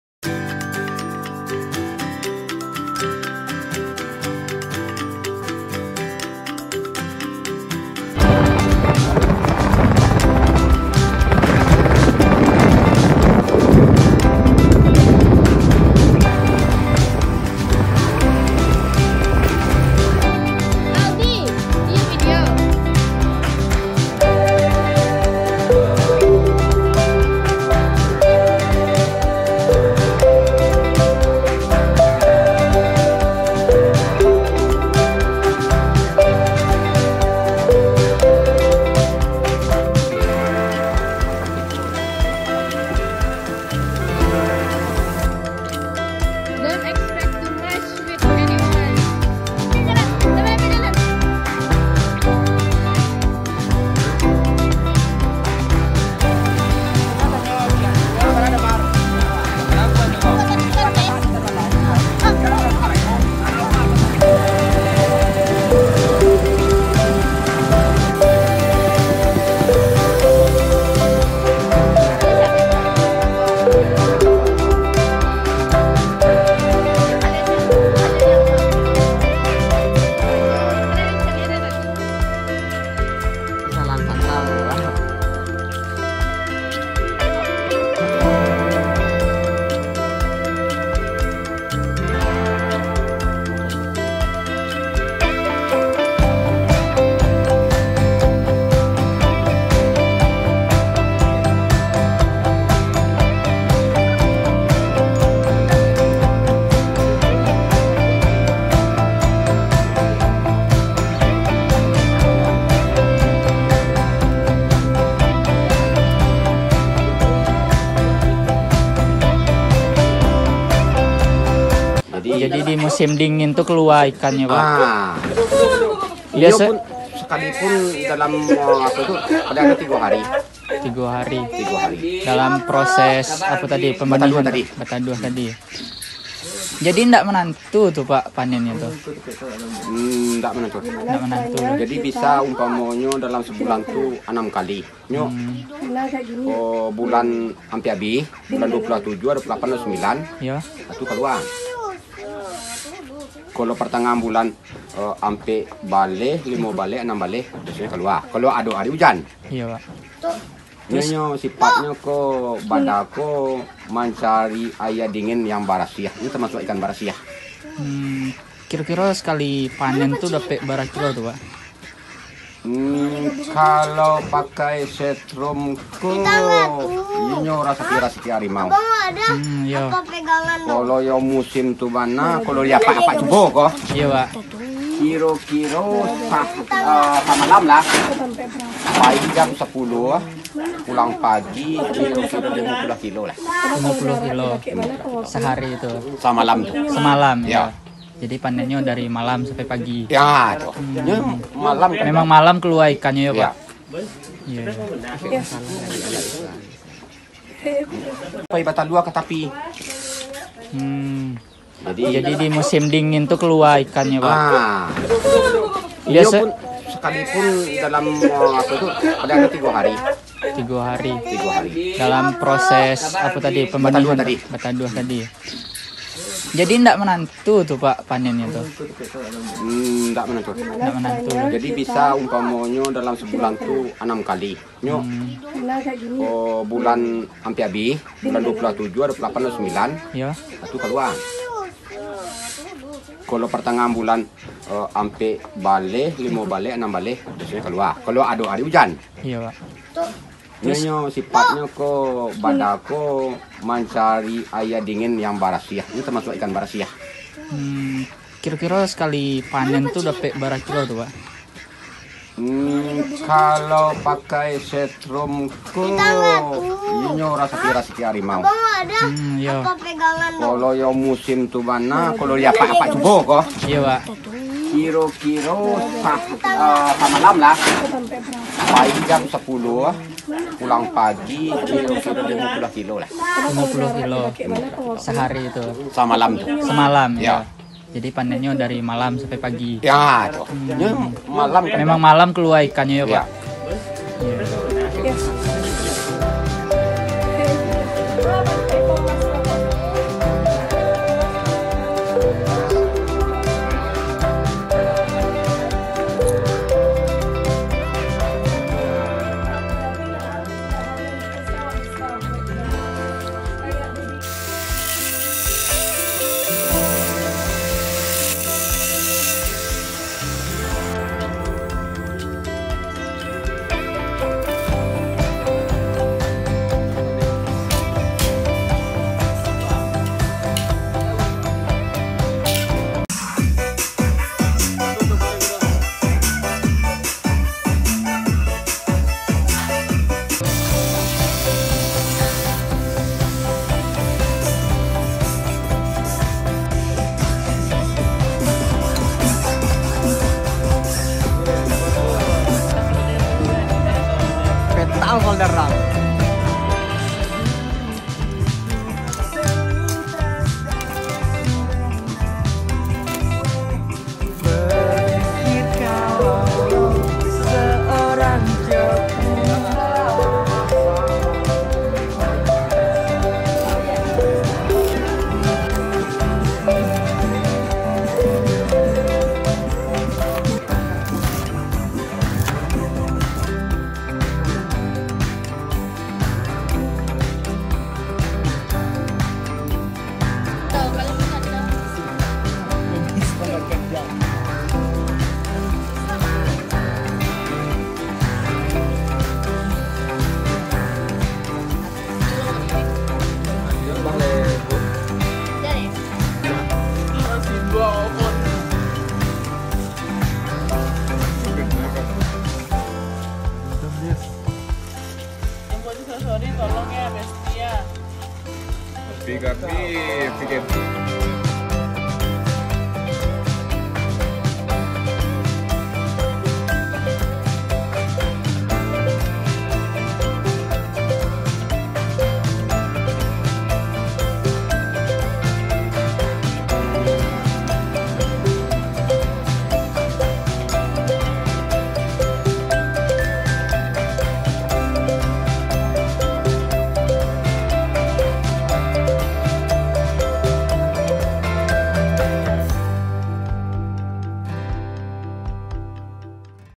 Program Pemberdayaan Masyarakat Desa (P2MD) "Ecotech Grow-Up" Universitas Negeri Padang
Merupakan Program Pemberdayaan Masyarakat Desa (P2MD) dari tim Teknik Elektronika Universitas Negeri Padang yang berjudul "Peningkatan Ekonomi Masyarakat Berbasis Green Teknologi (Ecotech Grow-Up) Melalui Perbaikan produksi Dan Menajemen Pemasaran Benih Ikan Nila Di Kabupaten Agam" yang berlokasi di Nagari Bayua Kecamatan Tanjung Raya Kabupaten Agam Provinsi Sumatera Barat.